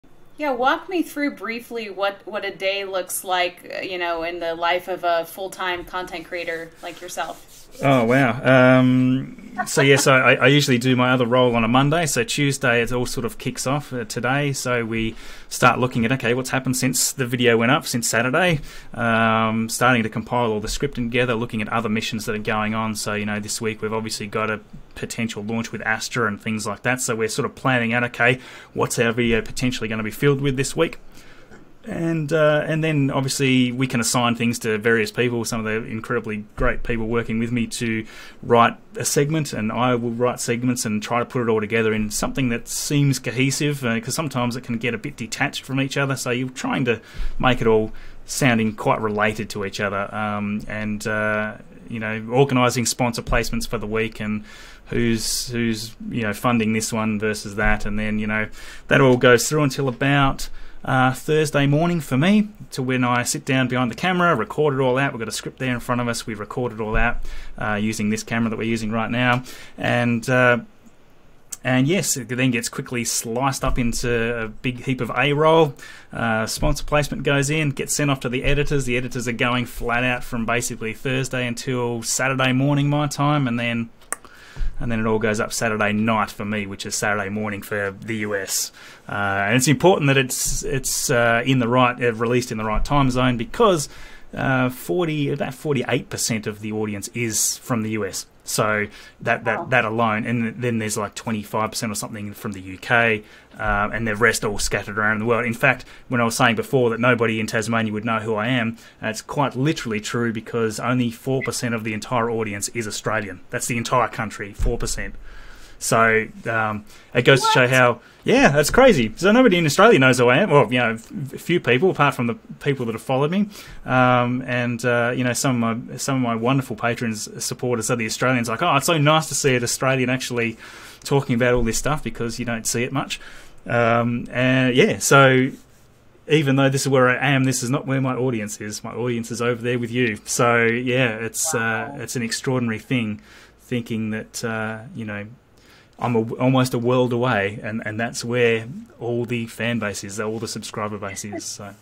CLIP: Marcus House shares weekly routine as a full-time YouTube creator
This is a clip from my in-depth interview with Marcus House. Marcus has a channel dedicated to SpaceX-related news. Many of YOU follow him, He is hands-down one of the nicest people I have interviewed and I can't wait to show you more of the unscripted version of our favorite "hey, hey" YouTuber.

HI! I am a news anchor and I have a passion for making videos and I love sharing parts of my experience with you, the viewer!